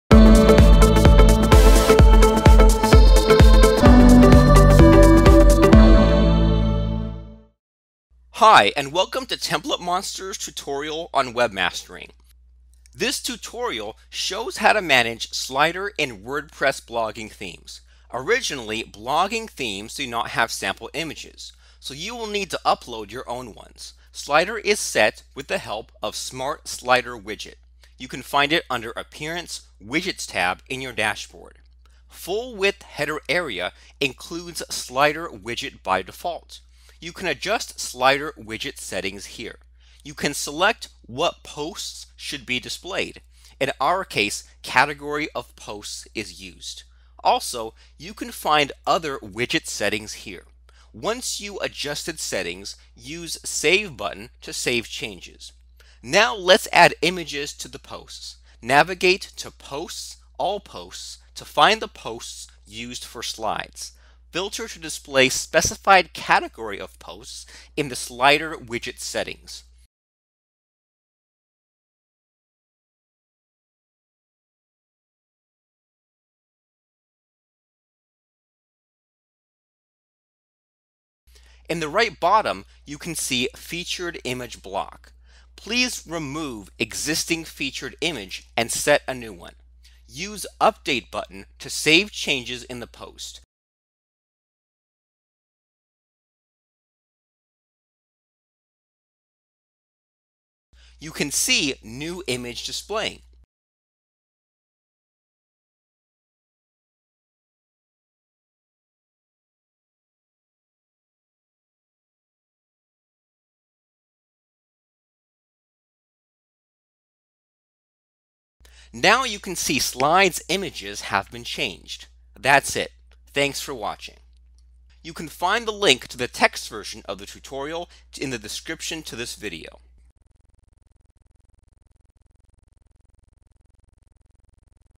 WordPress Blogging Themes. How To Manage Slider
This tutorial is going to show you how to manage slider.